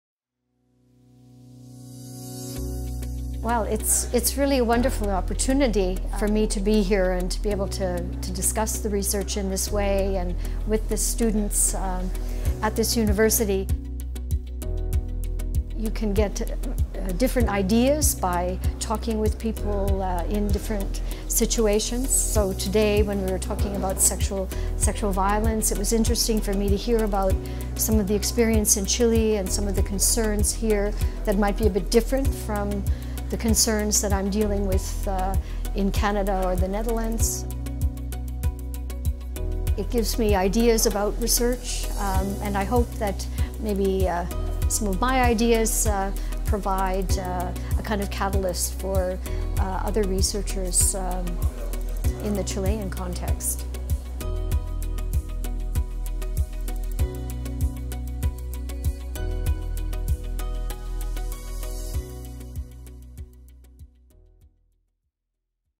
Summer School 2023 | Comentario Lorraine Radtke
Revisa el comentario de Lorraine Radtke, expositora del encuentro del Doctorado en Psicología de la Universidad Alberto Hurtado, sobre el Summer School 2023